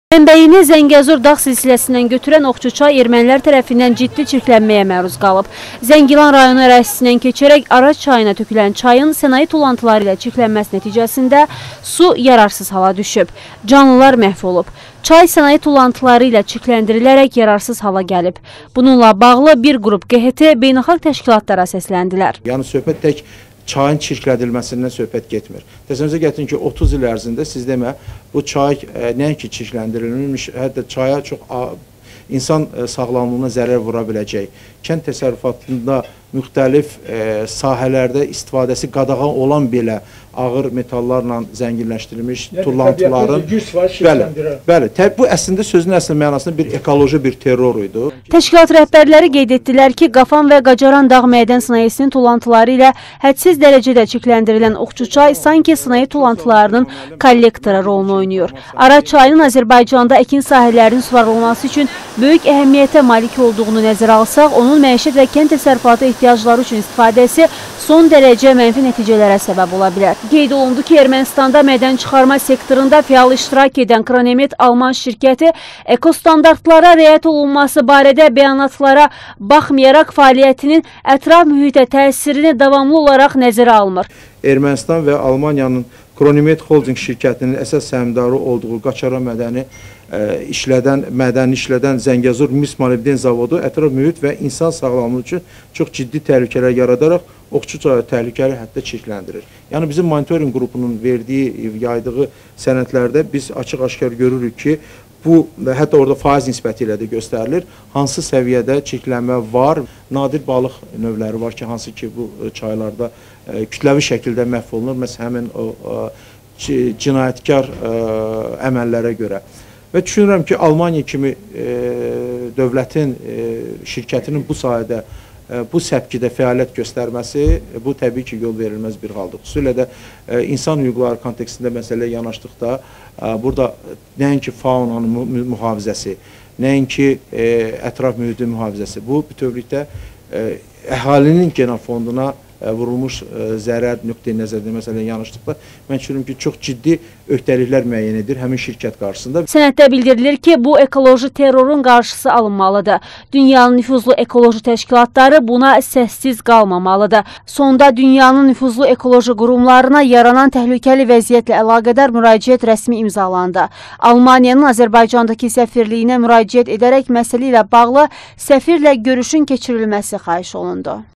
Oxçuçay təhlükəli həddə çirkləndirilib
Bir qrup QHT bununla bağlI beynəlxalq təşkilatlara müraciət etdilər